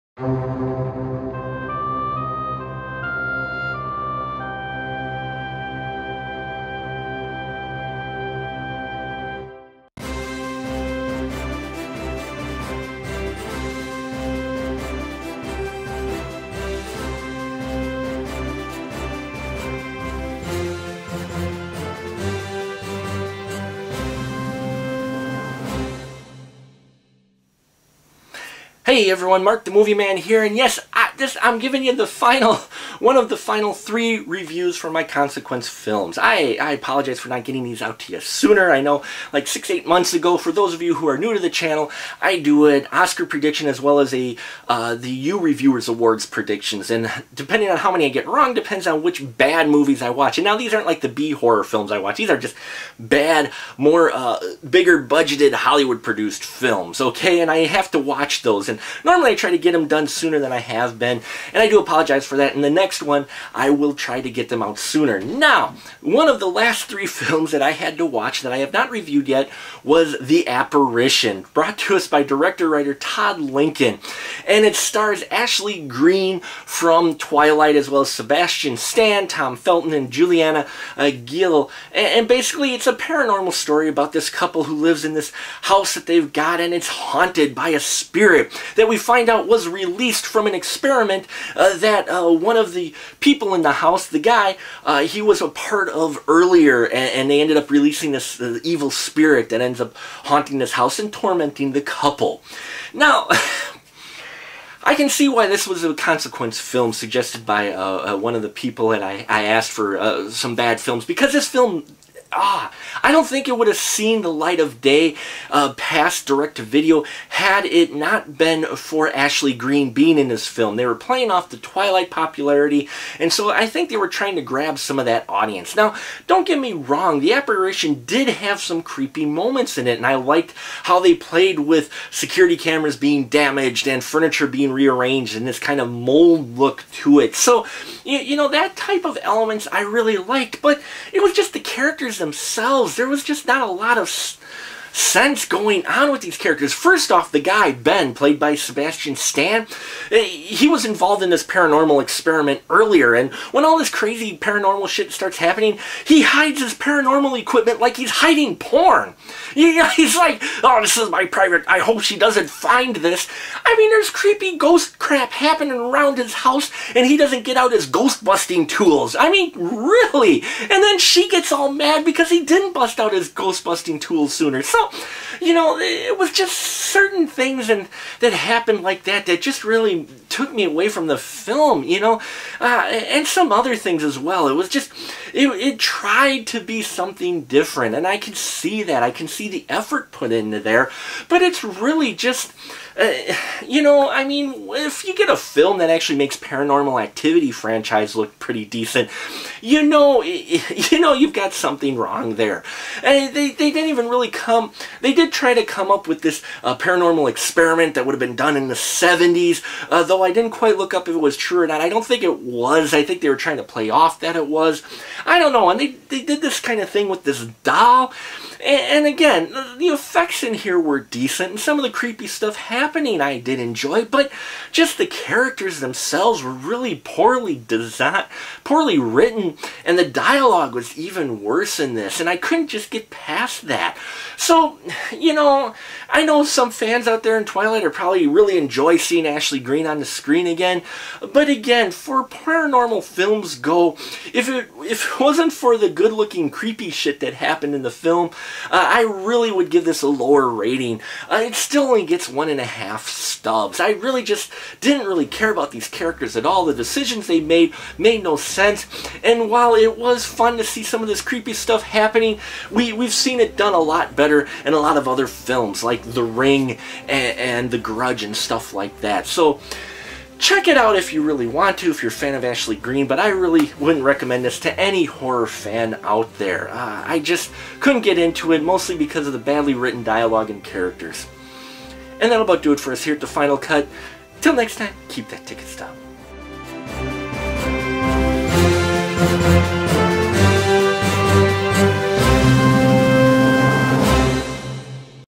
The Apparition (2012) Review on The Final Cut
Today we cover one of the last three consequence films I had to review for my fail at oscar and youtube reviewer predictions. We look at The Apparition. Next year I will get these done sooner. Really I will :D